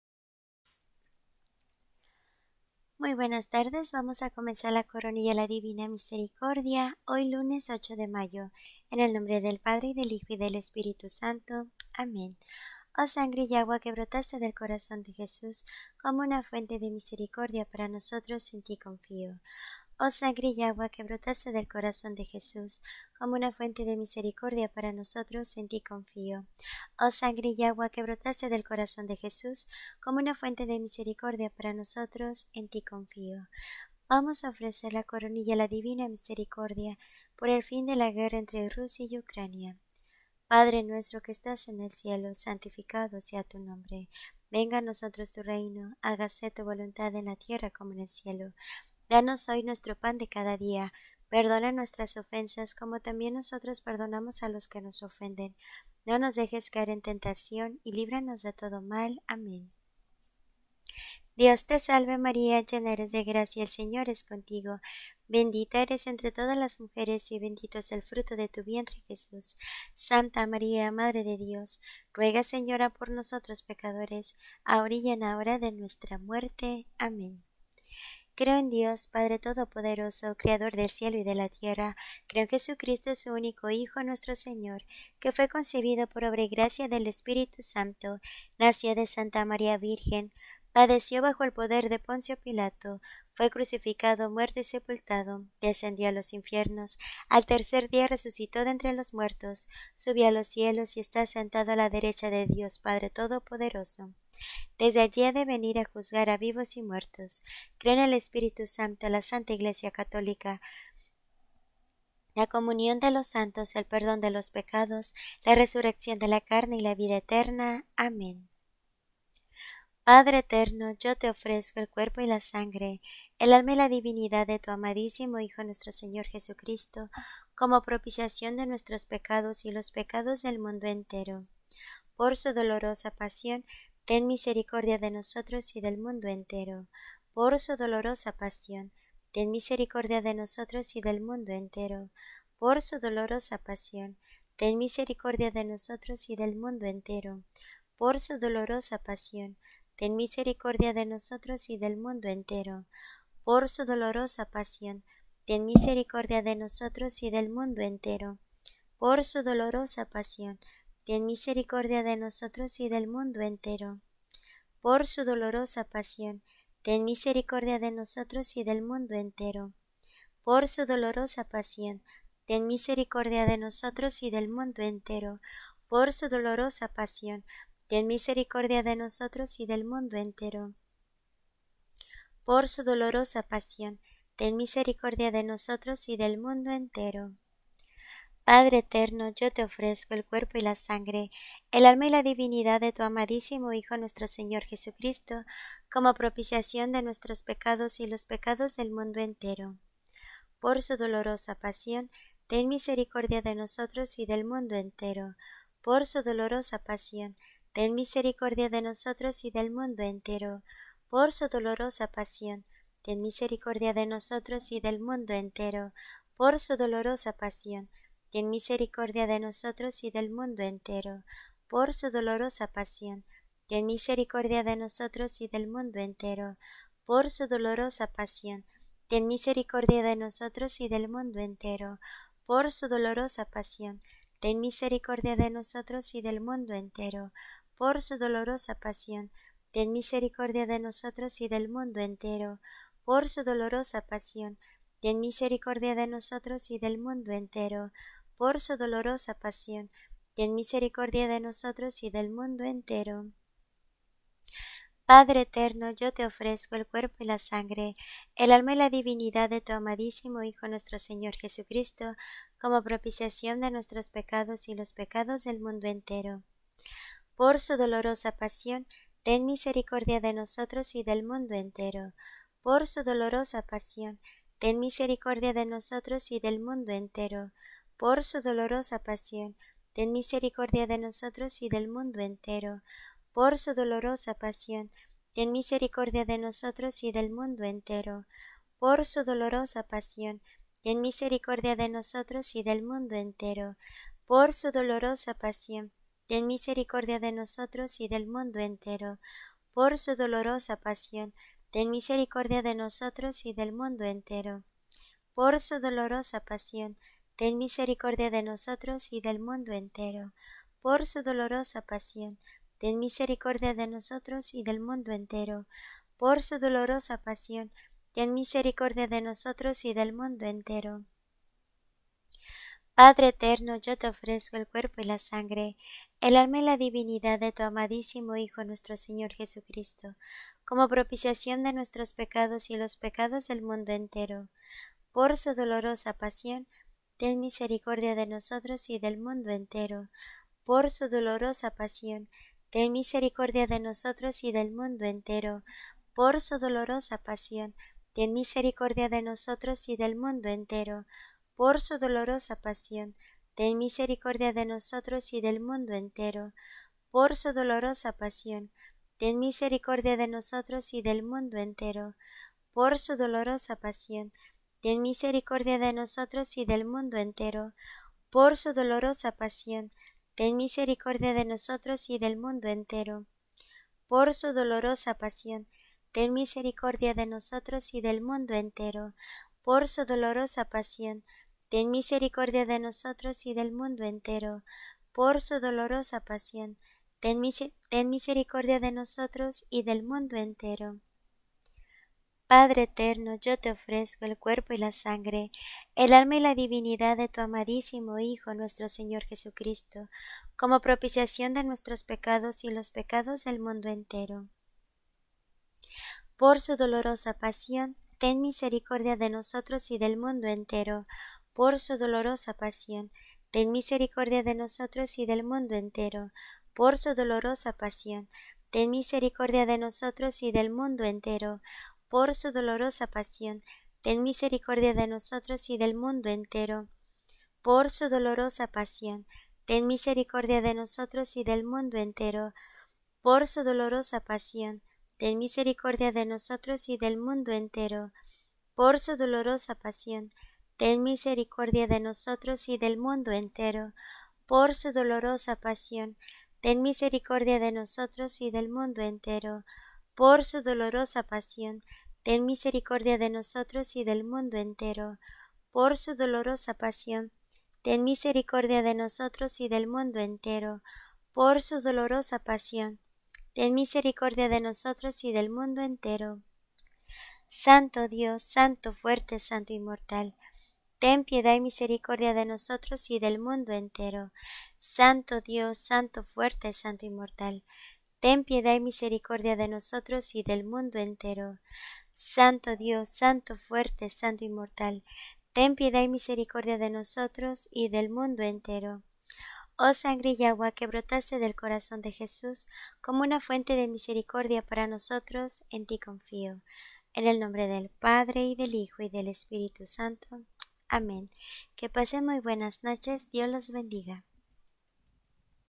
Coronilla a la Divina Misericordia 8 de mayo
Coronilla a la Divina Misericordia hoy 8 de mayo por el fin de la guerra entre Rusia y Ucrania.